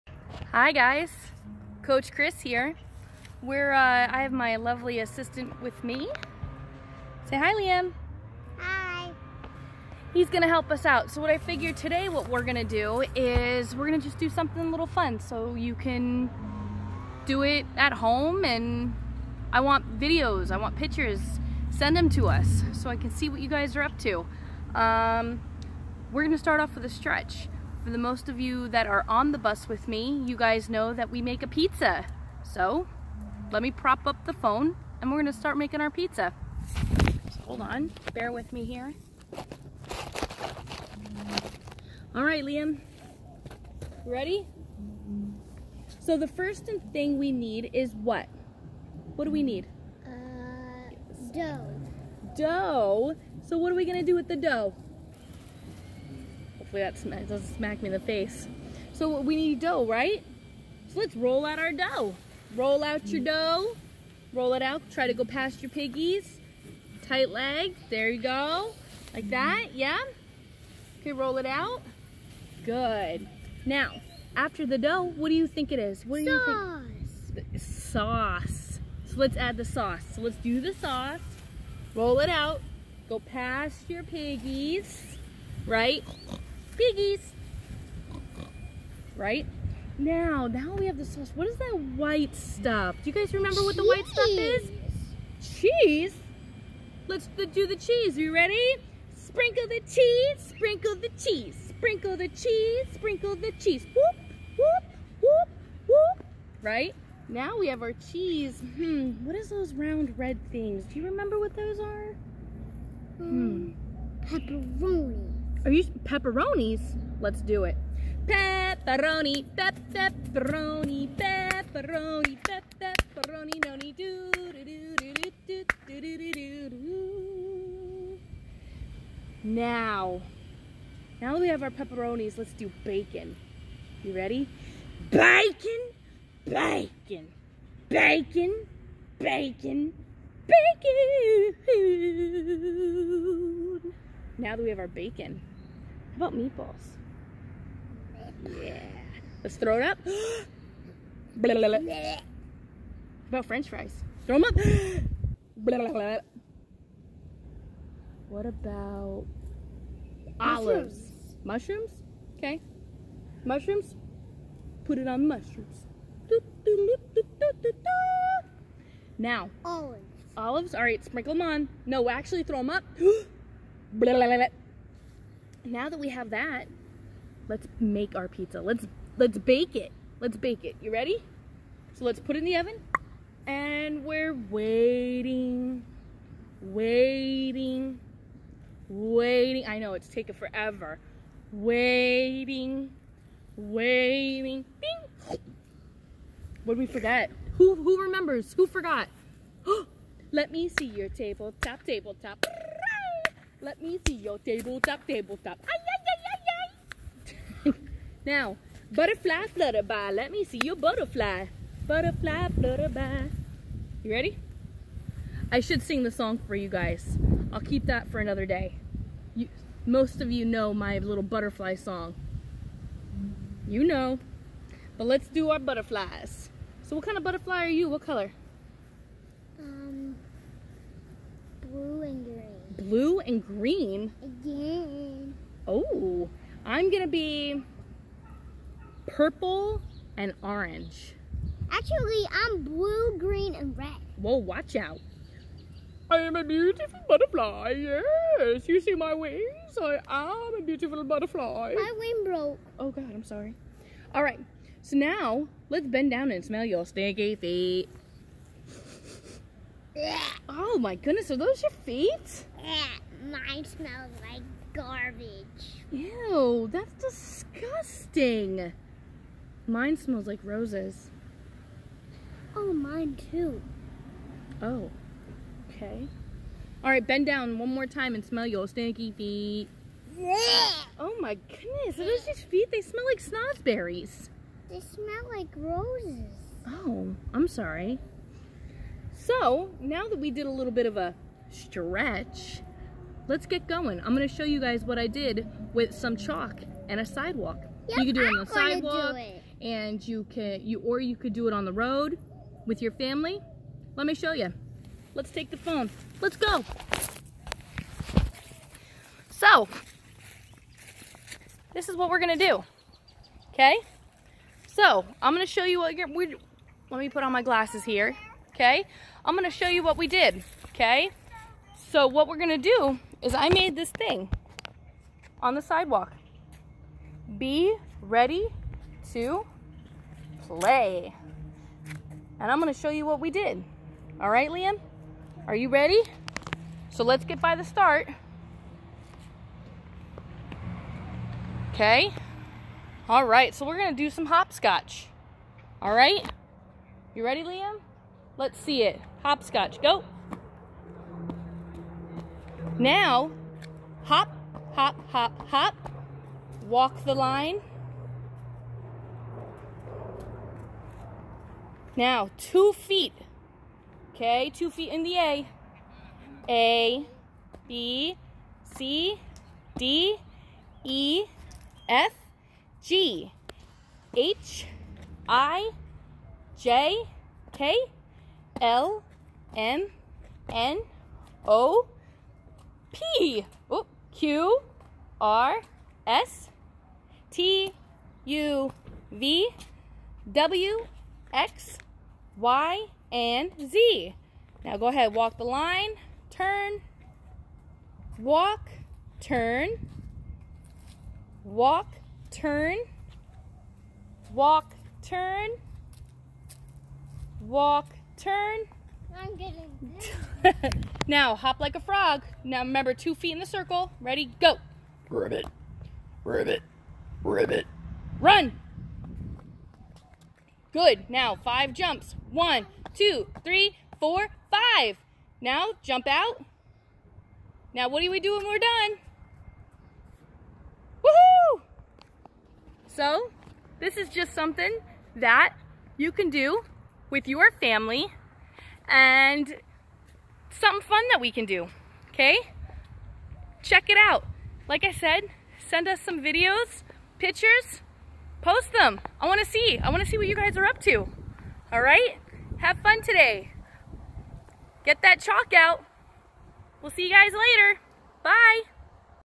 Tumblebus/AHGA/
Hi friends Coach Chris here! In this video, we started off with a little stretch we do on the Tumblebus. Also, I have my little assistant with me to show you guys what you can do with chalk! You can draw your own little obstacle course!! Have fun! Don't forget to send us your videos and or pictures of what you came up with!!! Miss you all